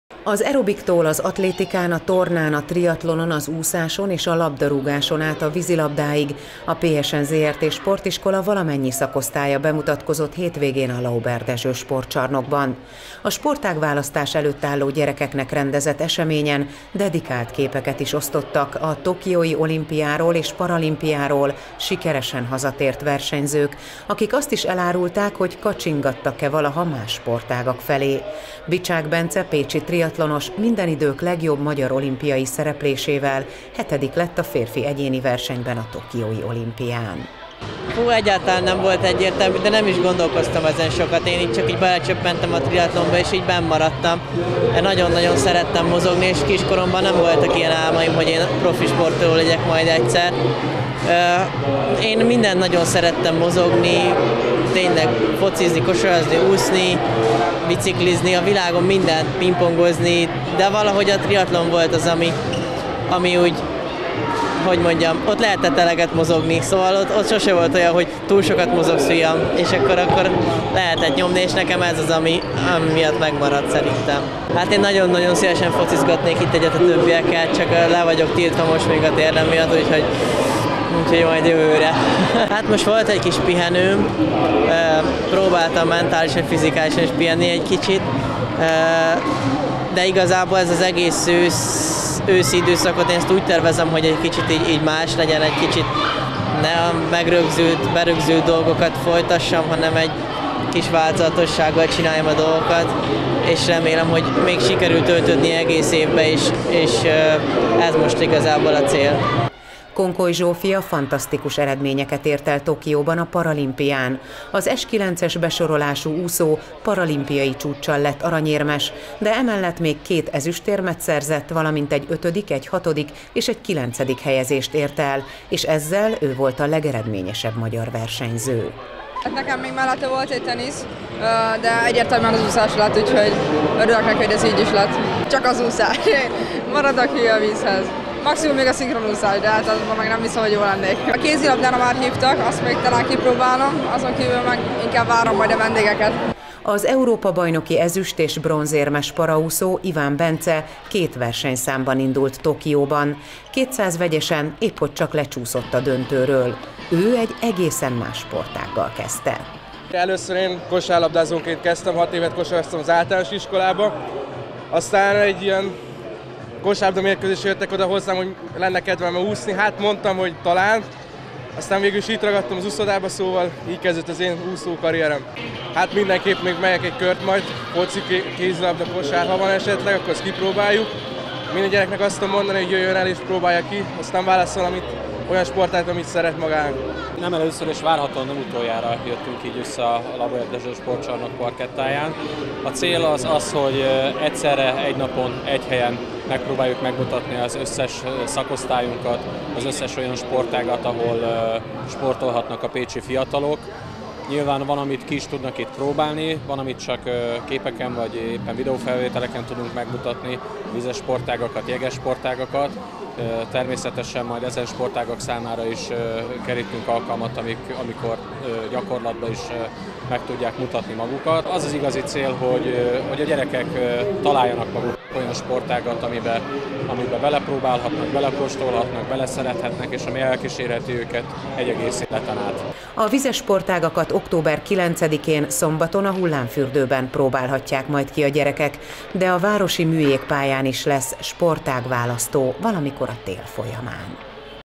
Sportágválasztó - Pécs
A PSN Zrt. Pécsi Sportiskola olimpikonjaival is találkozhattak, akik a PSN Zrt. Sportágválasztójára érkeztek szombaton. A paralimpiai bajnok Konkoly Zsófia mellett az Európa-bajnoki ezüst- és bronzérmes paraúszó, Iván Bence és a tokiói olimpián sporttörténelmi 7. helyet megszerző triatlonos, Bicsák Bence várta a gyerekeket a Lauber Dezső Sportcsarnokban.